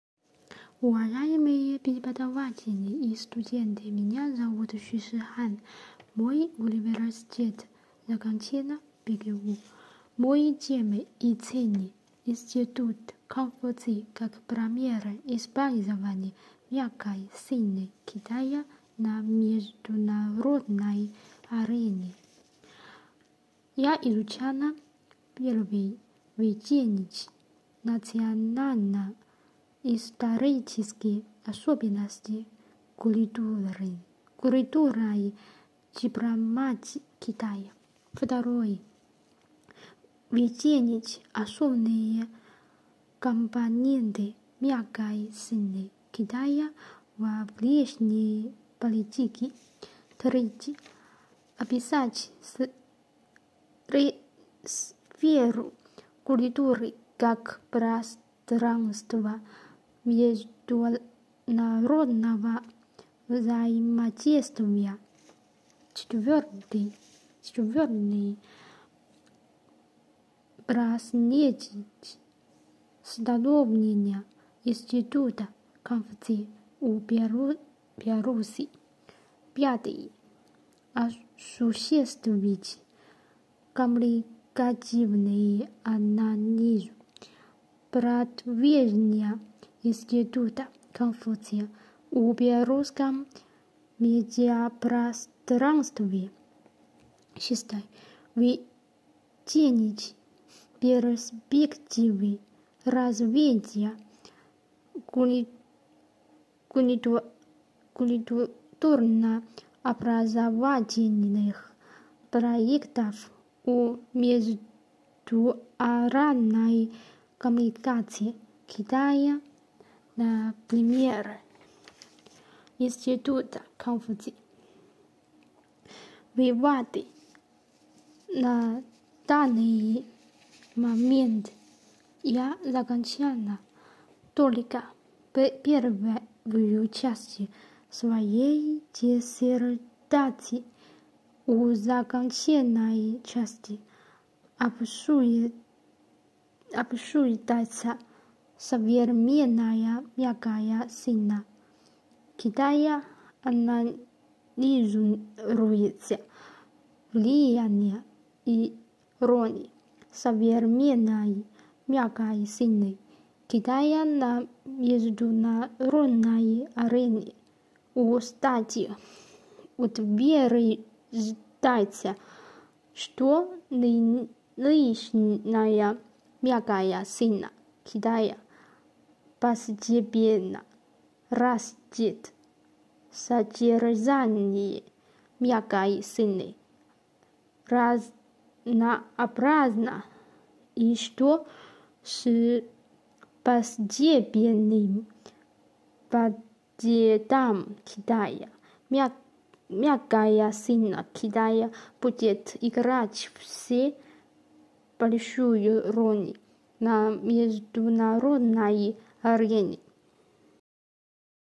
5_Сюй Шихань
Сюй Шихань
Институт Конфуция как пример использования «мягкой силы» Китая на международной арене
The Confucius Institute as an example of China's "soft power" in the international arena